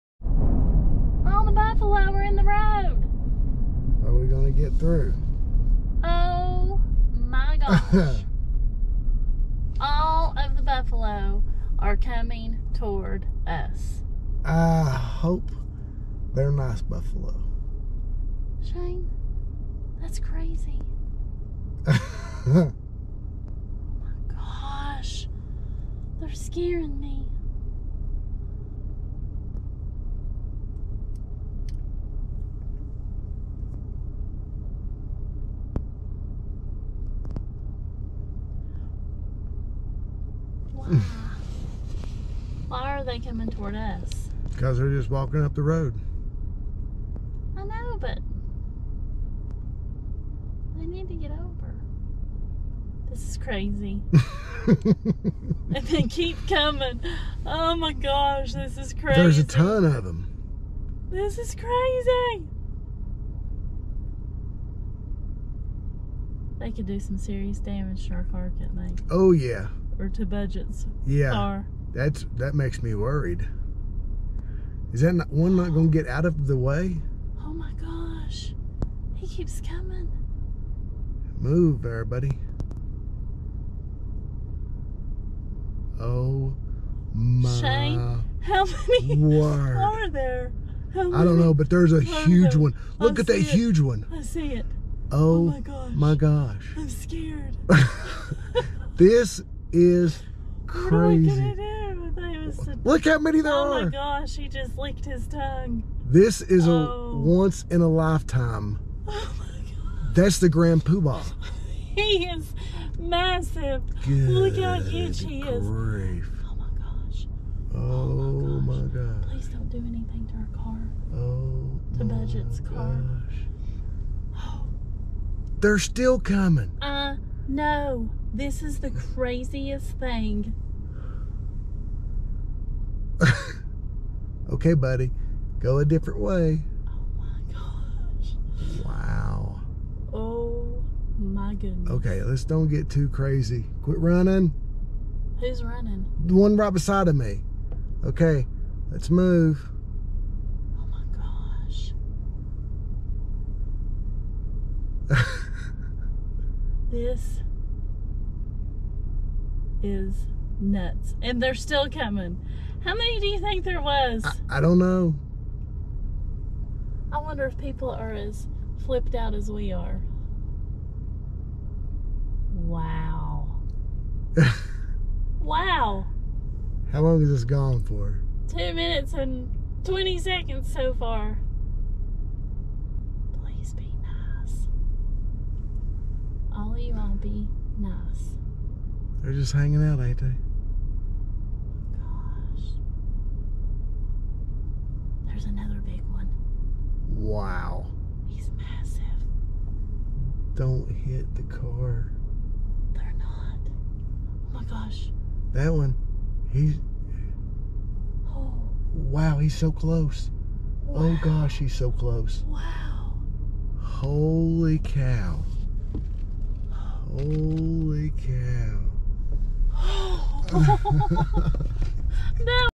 2nd camera Driver cam GoPro hero 9 Bison Jam Yellowstone national park over 150 *VIRAL*
Here is the second camera that was recorded by Melony Broyles on our trip to Yellowstone in October.  This is just another angle of the huge Bison Jam that we encountered.